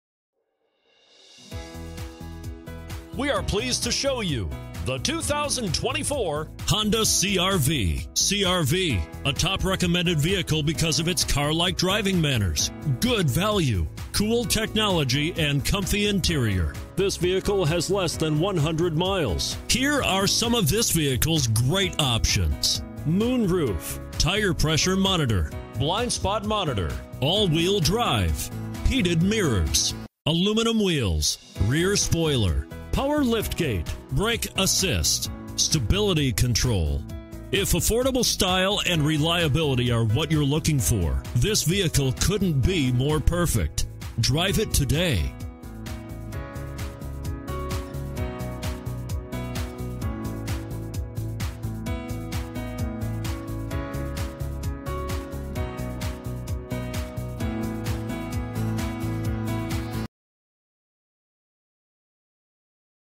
2024 Honda CR-V Nanuet NY RH476189A
2024 Honda CR-V EX-L

Crystal Black Pearl 2024 Honda CR-V EX-L AWD CVT 1.5L I4 DOHC 16VbrbrRecent Arrival! 27/32 City/Highway MPG